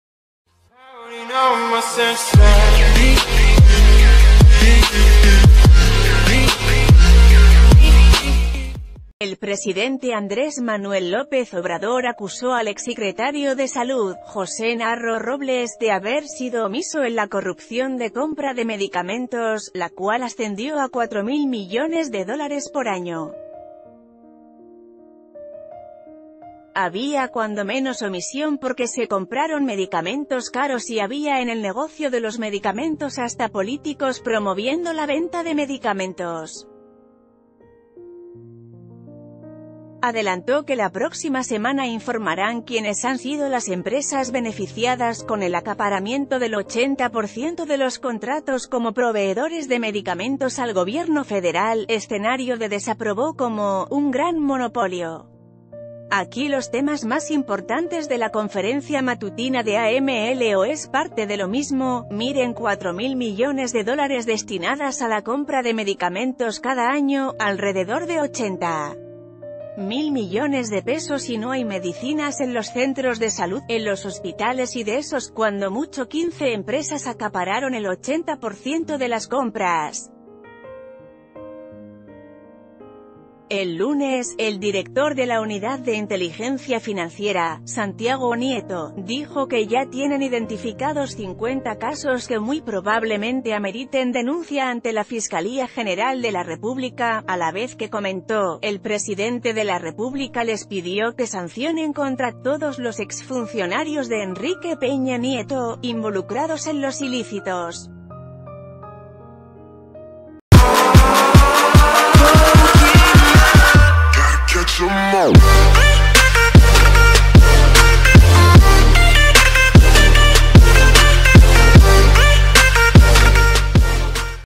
AMLO acusa a José Narro por permitir corrupción de compra de medicamentos
El presidente Andrés Manuel López Obrador acusó al exsecretario de Salud, José Narro Robles, de haber sido omiso en la corrupción de compra de medicamentos, la cual ascendió a cuatro mil millones de dólares por año. Había cuando menos omisión porque se compraron medicamentos caros y había en el nego...